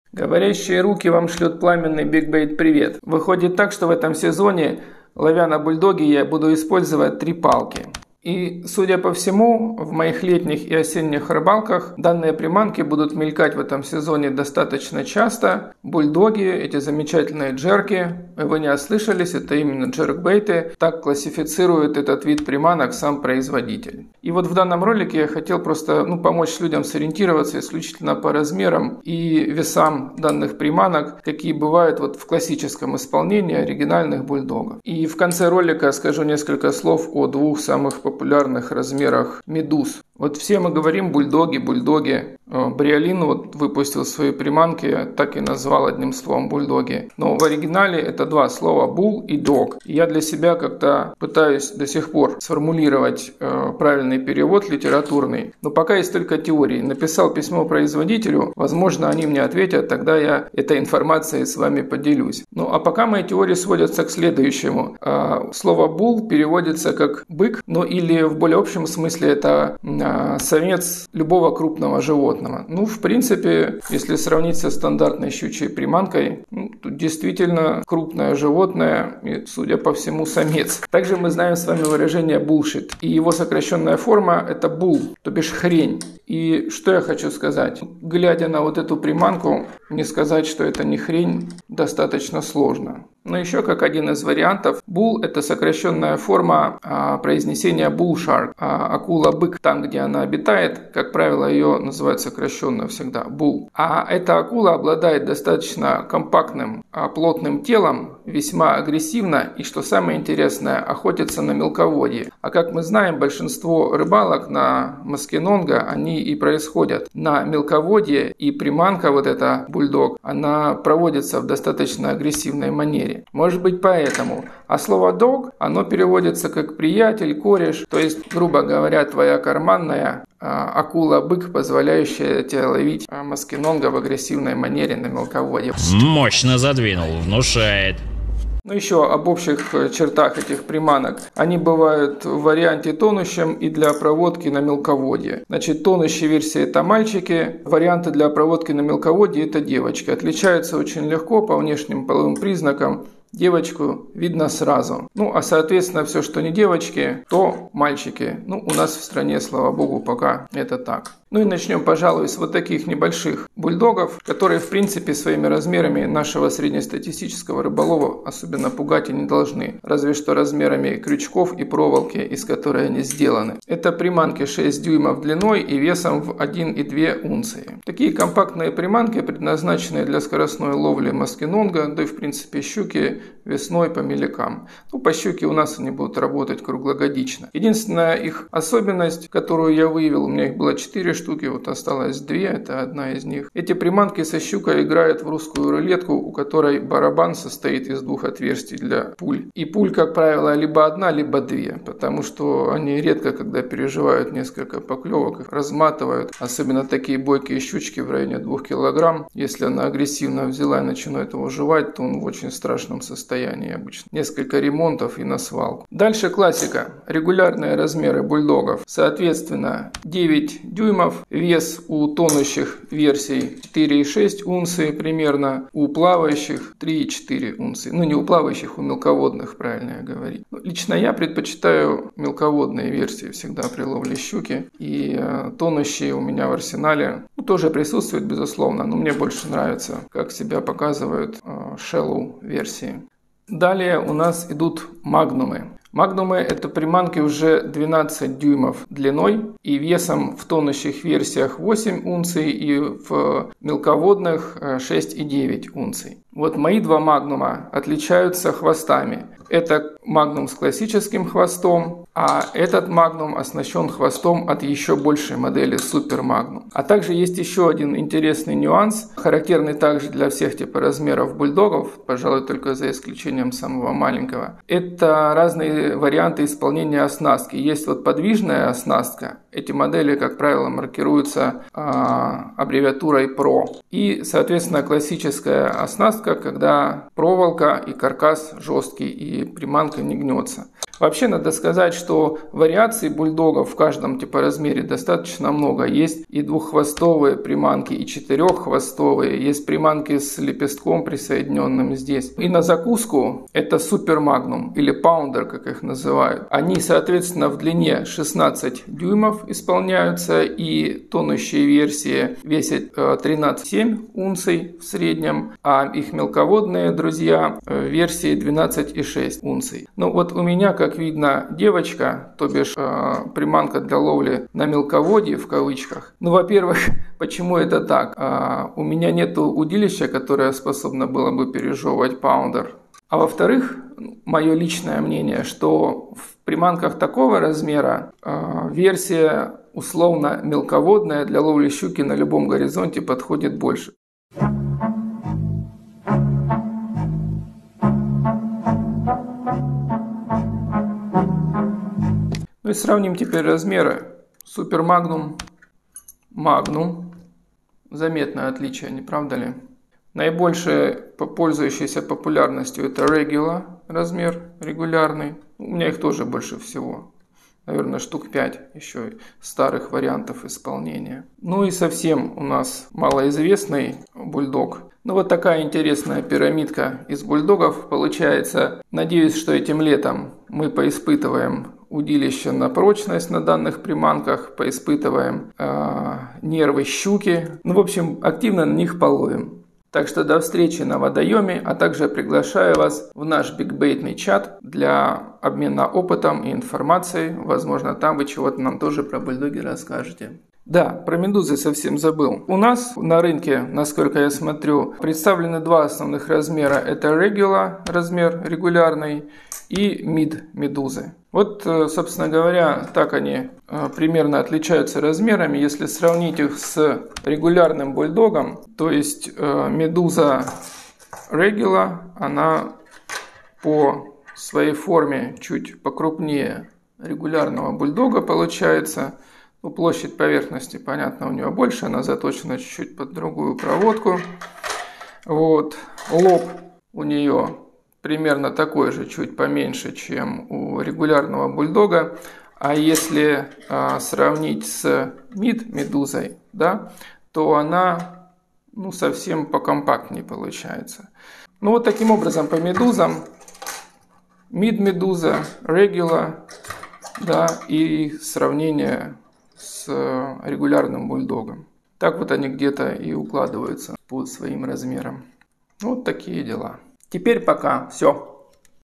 ВСЕ о размерах БУЛЬДОГОВ (BULL DAWG) и актуальных моделей МЕДУЗ (MEDUSSA) для ловли щуки. Сравнение.
Тайм-коды:
Джеркбейт Bull Dawg 00:00
Теория о присхождении названия Bull Dawg 00:39
О мальчиках и девочках 02:18
Musky Innovations Spring Bull Dawg 02:41
Musky Innovations Regular Bull Dawg 03:36
Musky Innovations Magnum Bull Dawg 04:03
Musky Innovations Super Magnum Bull Dawg 05:02
Musky Innovations Monster Magnum Bull Dawg 05:44
Визуальное сравнение всех моделей 05:52
О Медузах (Chaos Tackle Medussa) 06:40